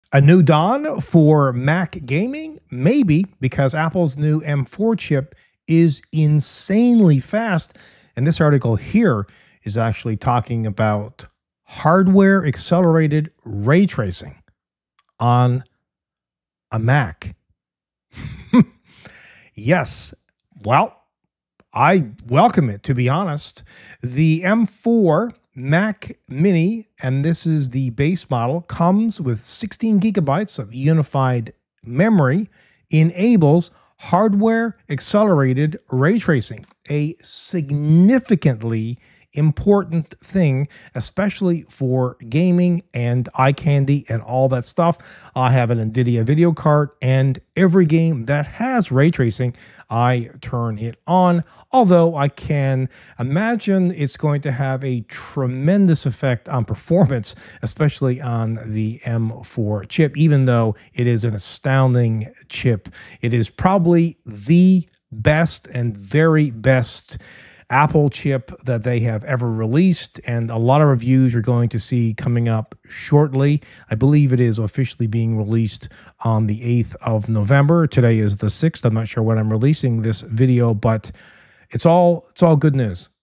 Mac Mini M4 Supports Ray Tracing
Apple's new M4 Mac mini, with its powerful M4 chip and hardware-accelerated ray tracing, has the potential to disrupt the gaming PC market. Here's a breakdown of its key features and implications:

Key Features:
M4 Chip: Offers significant performance improvements over previous generations.
Hardware-Accelerated Ray Tracing: Enables advanced graphics capabilities for gaming.
16GB Unified Memory: Provides ample memory for both system and graphics tasks.
Competitive Pricing: The base model is priced attractively at $599.
Implications for the Gaming Market:

Challenging Traditional Gaming PCs: The M4 Mac mini's performance and pricing could make it a compelling option for gamers, especially those who prioritize a compact and efficient system.
Expanding Gaming Library: Apple's efforts to bring popular games to the Mac platform, such as Resident Evil 4 and Cyberpunk 2077, further solidify its position in the gaming market.
While the M4 Mac mini may not compete directly with high-end gaming PCs, it offers a compelling alternative for gamers who value performance, efficiency, and a sleek design.